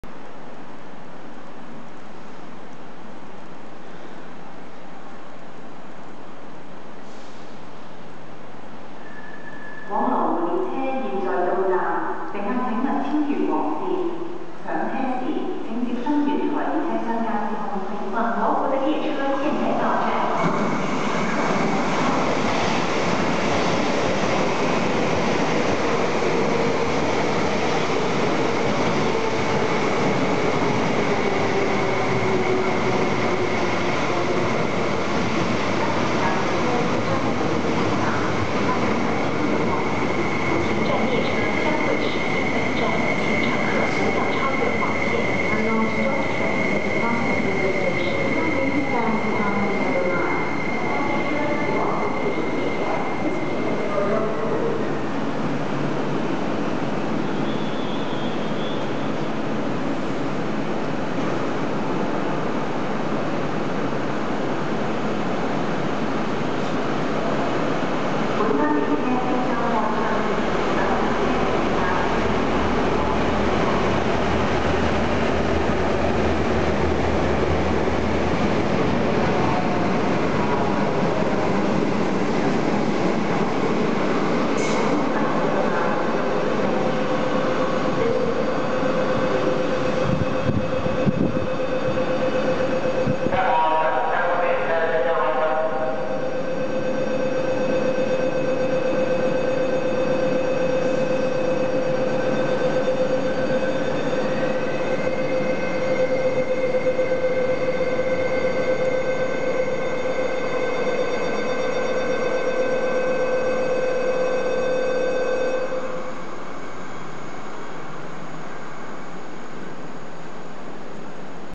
港鐵柴油機車62+58號牽引貨運列車駛經港鐵旺角東站
拍攝日期:26/12/2009 加入日期:26/12/2009